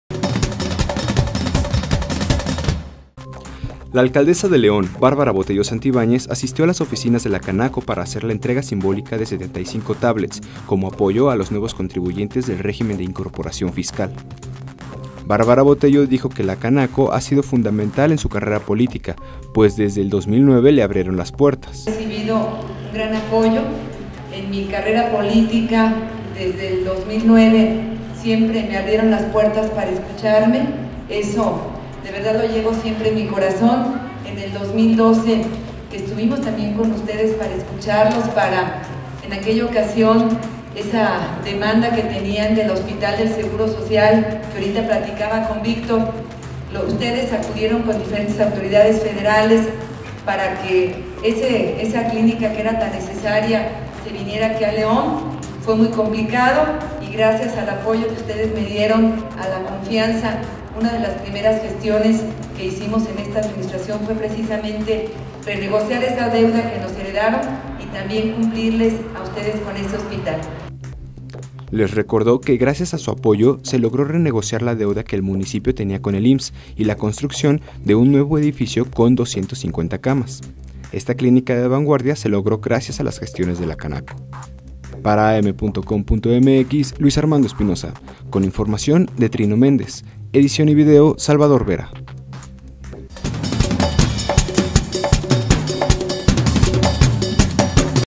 Bárbara Botello entrega 'tablets' a Canaco
La alcaldesa Bárbara Botello Santibáñez, asistió a las oficinas de Canaco para hacer la entrega simbólica de 75 “tablets” para apoyo a los nuevos contribuyentes del Régimen de Incorporación Fiscal (RIF). Fue recibida por el ex presidente de la Canaco, José Luis Sandoval Miranda.